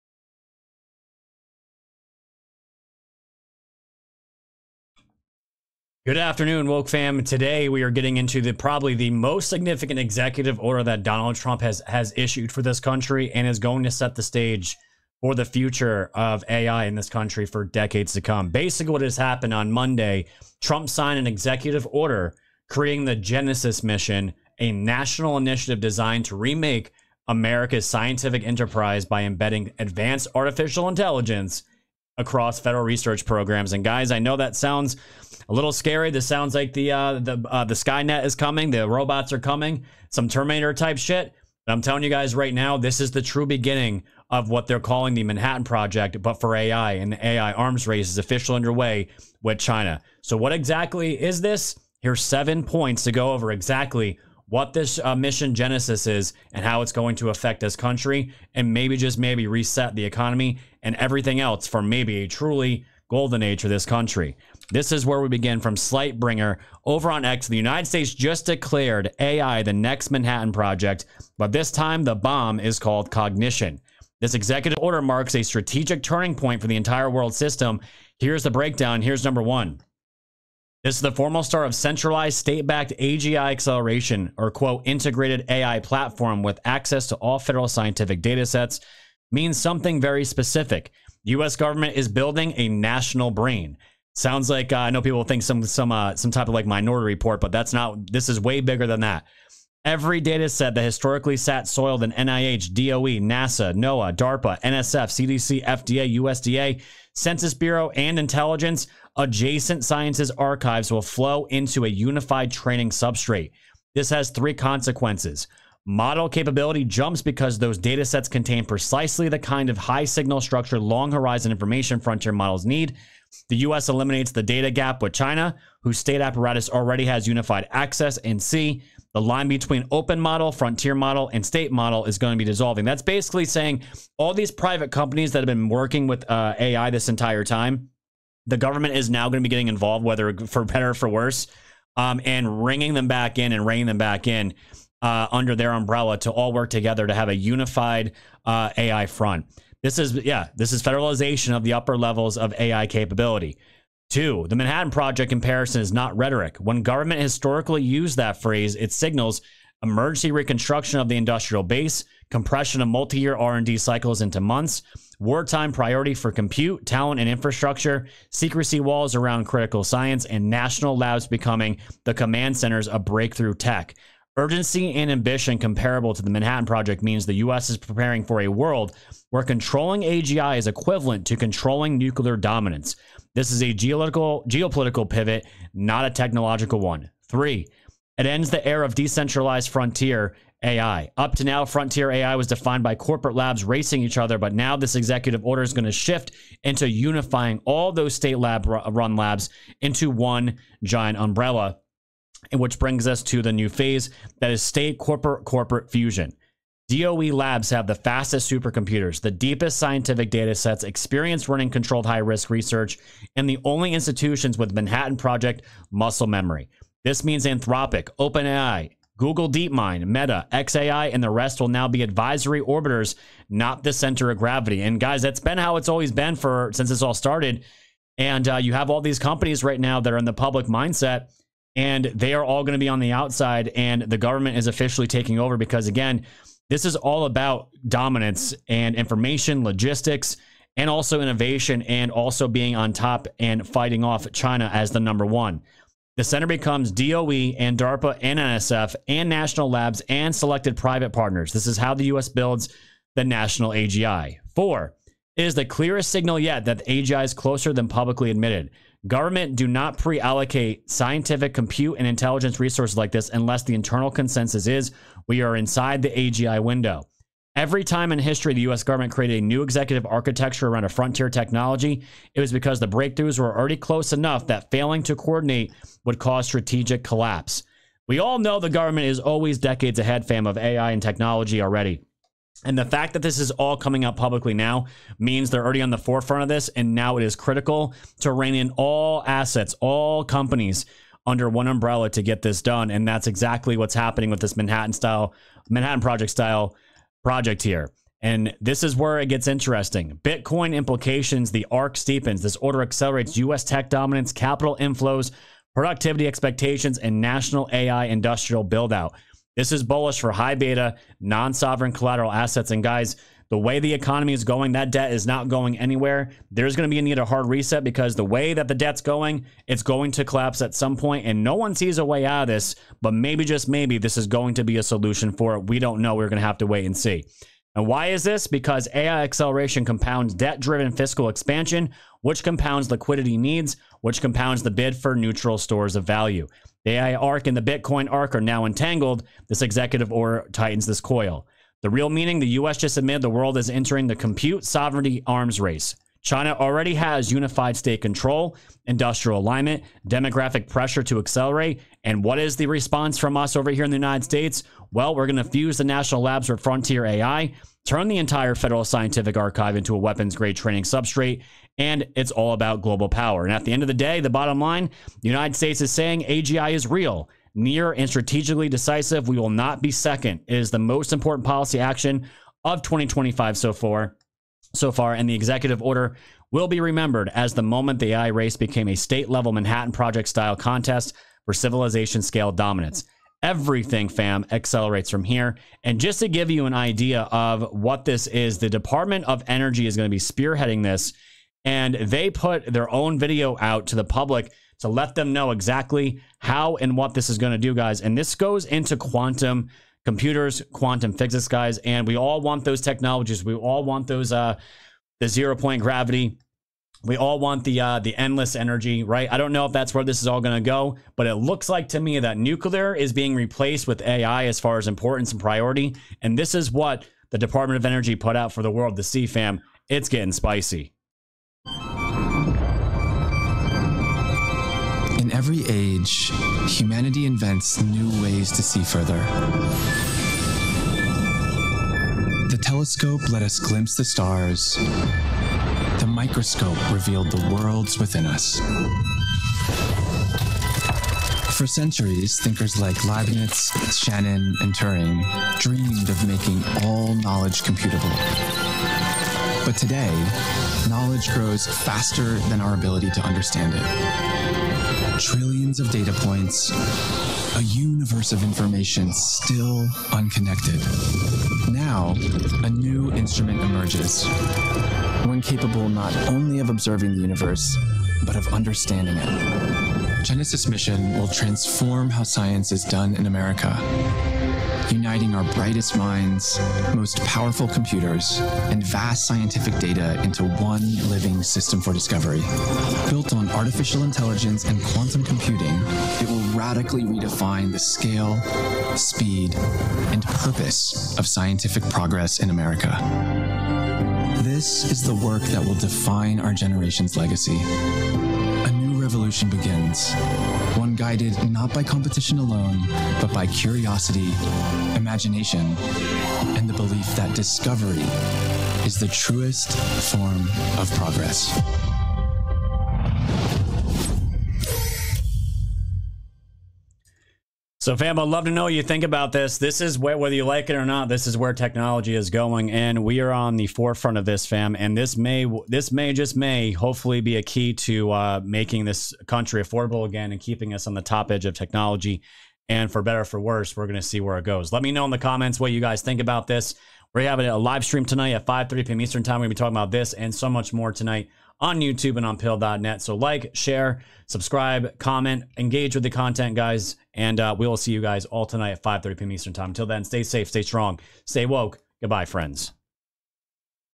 Genesis Mission EXPLAINED: The Executive Order Building America’s National AI Brain
Today’s video breaks down the newly emerging Genesis Mission — a sweeping U.S. initiative embedded inside the recent AI Executive Order that effectively launches the foundations of a national AI brain.

This “Genesis Mission” restructures the entire American scientific, intelligence, and technological landscape by:

Integrating AI across every major federal agency — NIH, DOE, DARPA, NASA, NOAA, CDC, FDA, NSF, Census, USDA, and more.

Fusing decades of siloed government datasets into a unified national AI system.

Transitioning from private frontier-model control to state-level AGI development.

Establishing a federal AGI strategy that includes compute allocation, model training, biosecurity modeling, climate simulations, nuclear stability forecasting, and strategic national planning.

Centralizing AI oversight, risk assessment, and real-time data ingestion under one coordinated mission architecture.

Shifting global power, positioning the United States for AI-driven dominance.

The Genesis Mission isn’t just a policy change — it’s the beginning of a state-run intelligence engine and the end of decentralized AI as we know it.